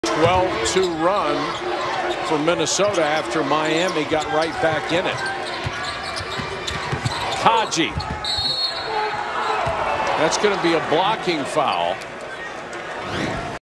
Block Charge186
On this play we see a great job of coverage by the crew.  The trail official initially begins officiating the drive to the basket.  He steps down with the play and stays connected as the offensive player goes by the primary defender.  At that time, the secondary defender moves from the leads primary coverage area and inserts himself into the play causing a crash at the basket.  The Trail is patient and allows the lead to have the whistle on this play.  Refereeing the defender we can see that he fails to establish LGP in the path of the offensive player prior to contact.  A blocking foul is correctly ruled by the lead official.